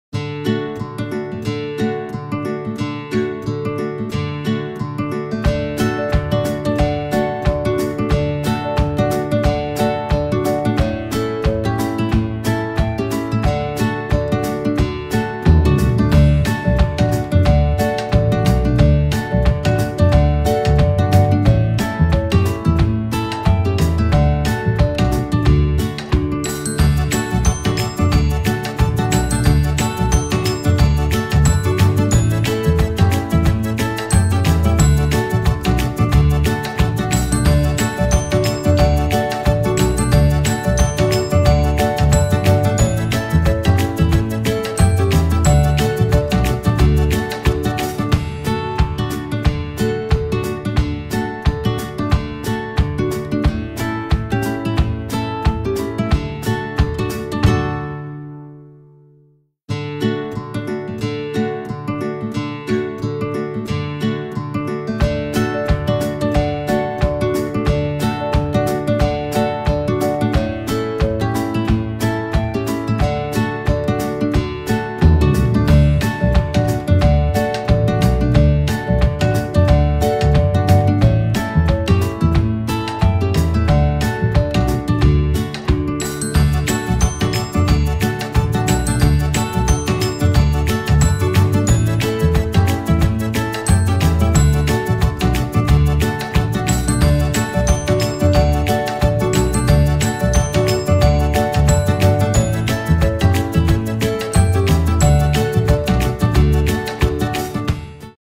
Embossed Phrases on Cookies - Creating with Cake In The Afternoon Cookie Designer
Creating Embossed cookies with Cake In The Afternoon Cookie Designer